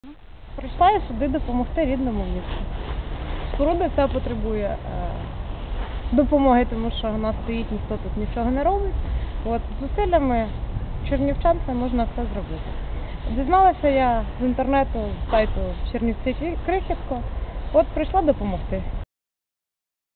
Волонтери впорядковували храм у Чернівцях
Волонтери прибирали костел Серця Ісуса у Чернівцях